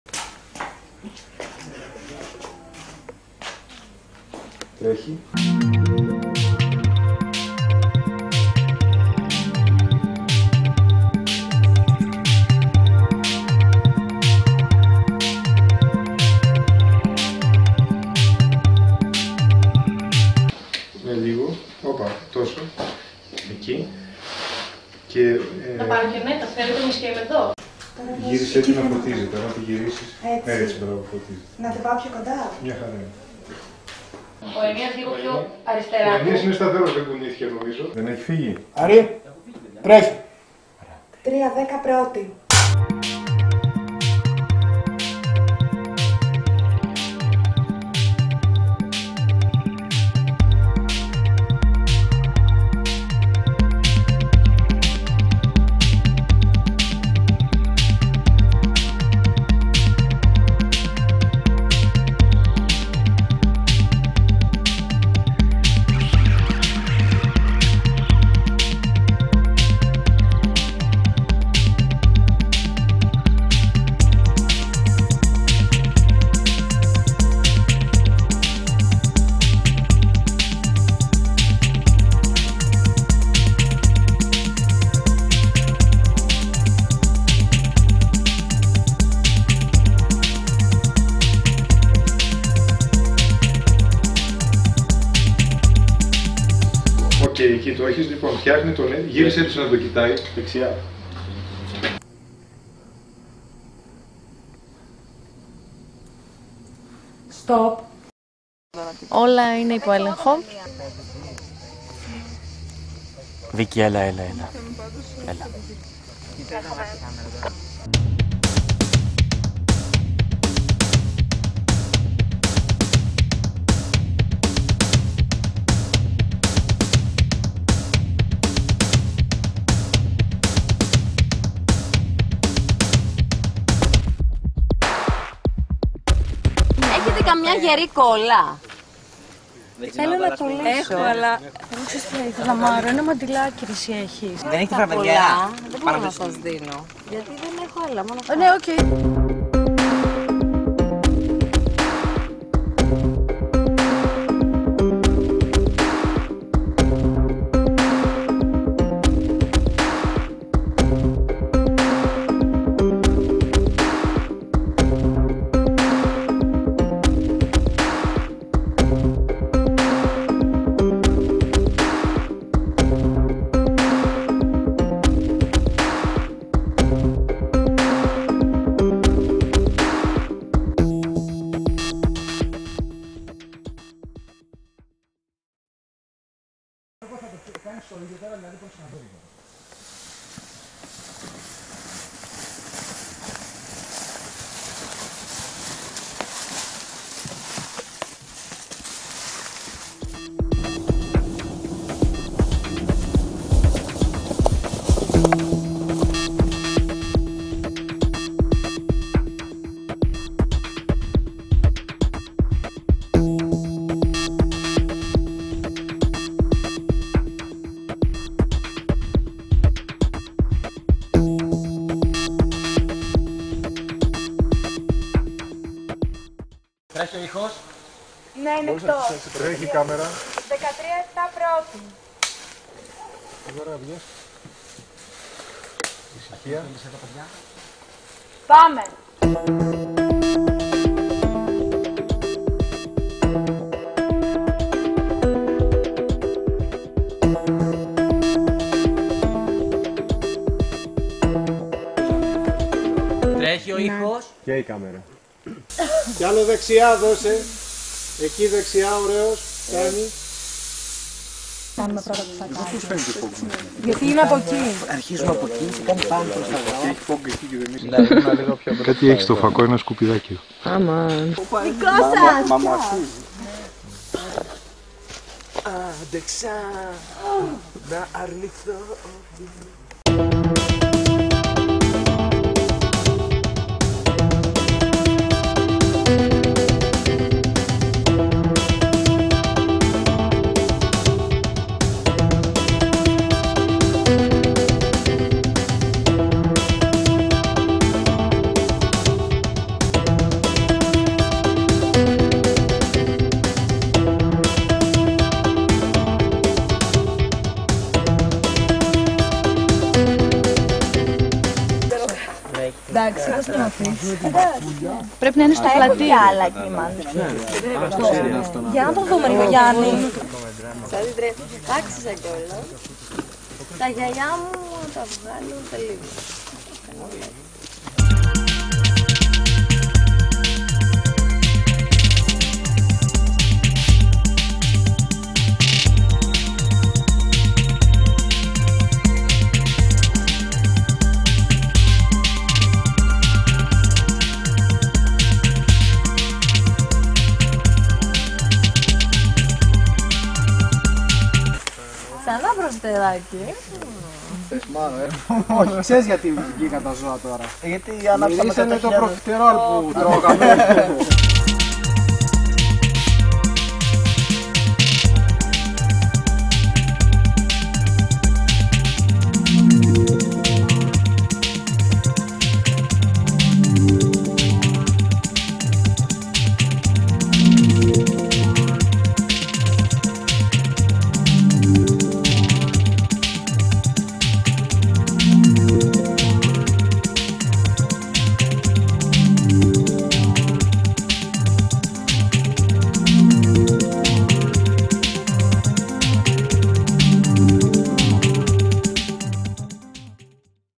short film ''Tholos'',ARISTOTLE UNIVERSITY OF THESSALONIKI-2011,making of
making of της ταινιας μικρου μηκους "Θόλος'' (τμημα κινηματογραφου ΑΠΘ)
Βασικοι συντελεστες της ταινιας:
Παραγωγη:Δεσποινα Σιφνιαδου
Σκηνοθεσια:Νικος Αποστολοπουλος,Κατερινα Γεροθαναση
Σεναριο:Ελιζαμπεττα Ηλια-Γεωργιαδου
Φωτογραφια:Γιαννης Δασκαλοθανασης
Μονταζ:Νικος Αποστολοπουλος
Σκηνογραφια,Ενδυματολογια:Χρυσα Σερδαρη
Ηχος,μιξαζ:Αρης Αθανασοπουλος
Παιζουν : Αινειας Σταματης , Γιωργος Καφετζοπουλος